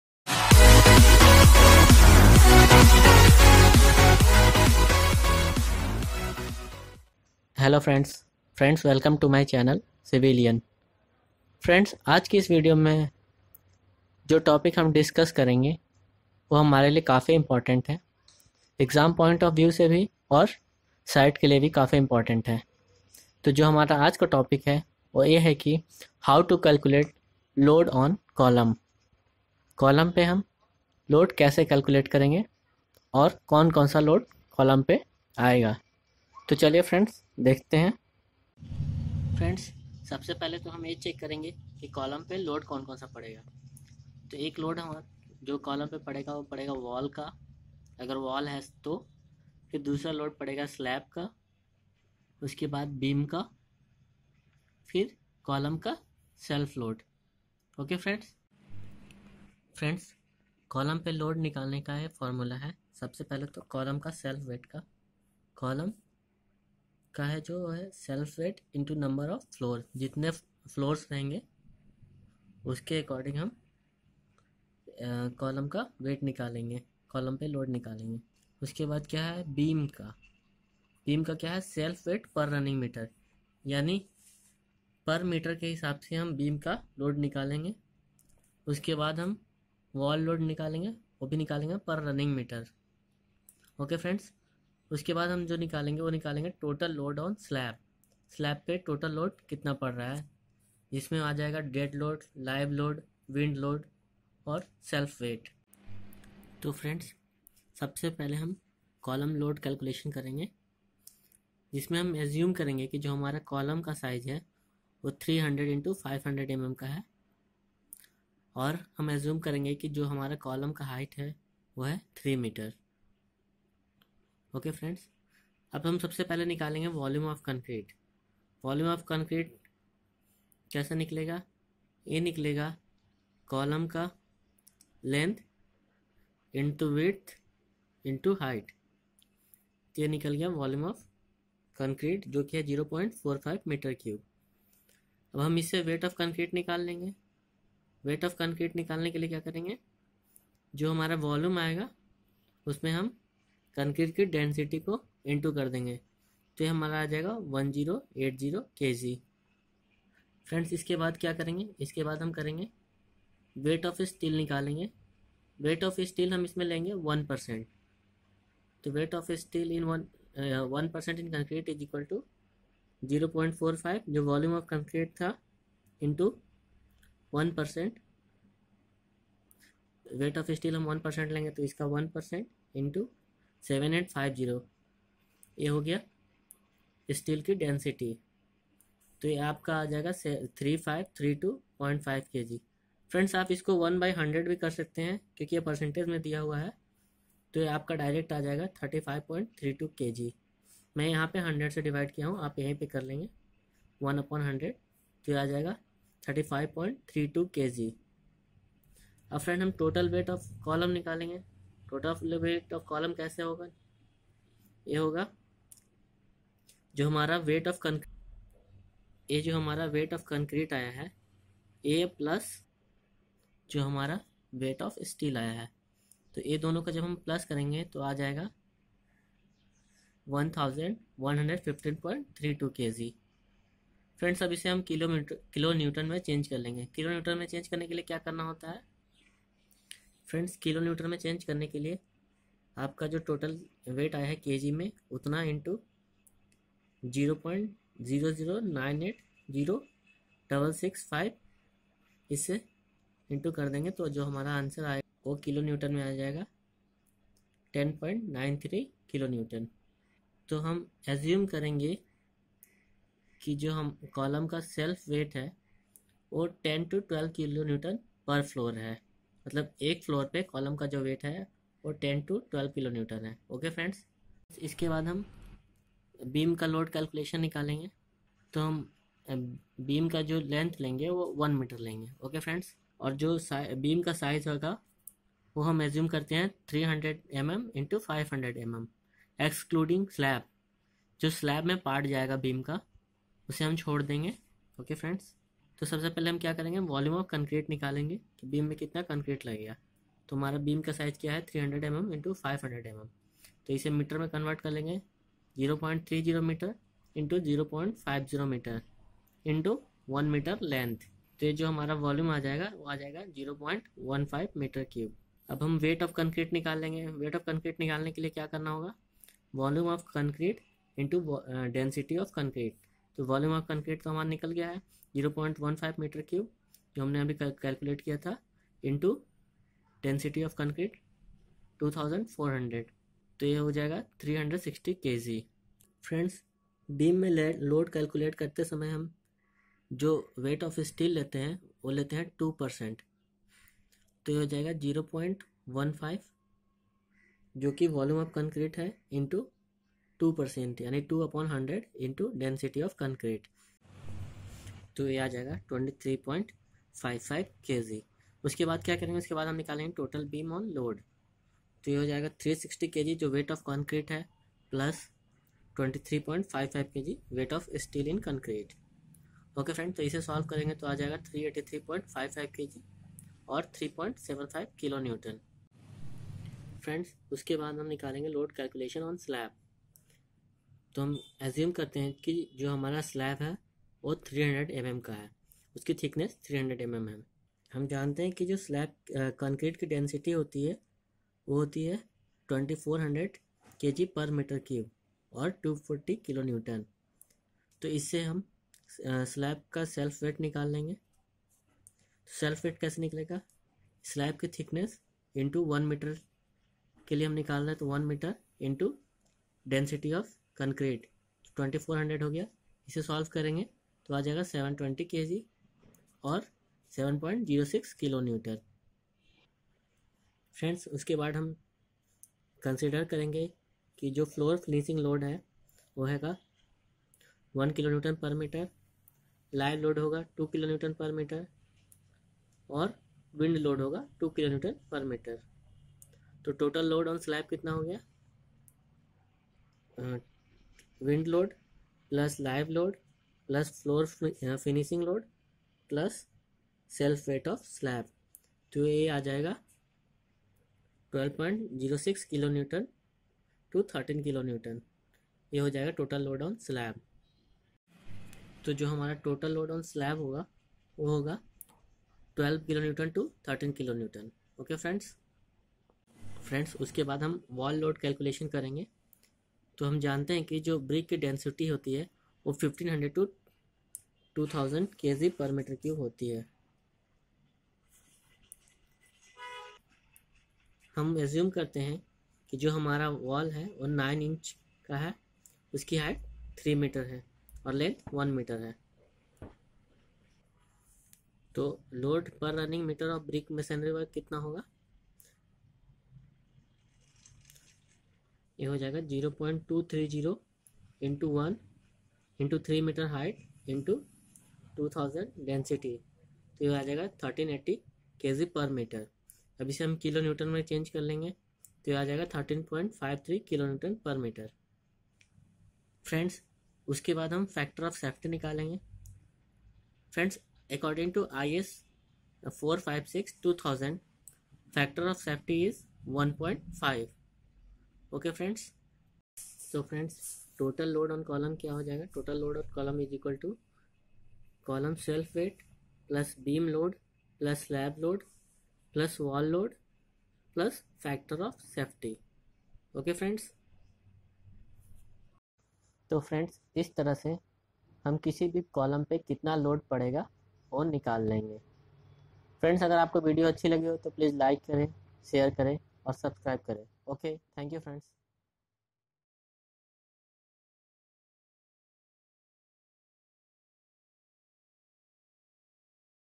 Load Calculation On Column, Beam, Slab// Column Self Weight
This video contain Load Calculation On Column.

Topic covered in this video⬇️

1) Column Self Weight
2) Beam load calculation
3) Slab load calculation
4) Wall load calculation
5) Factor of Safety
6) Total load on Column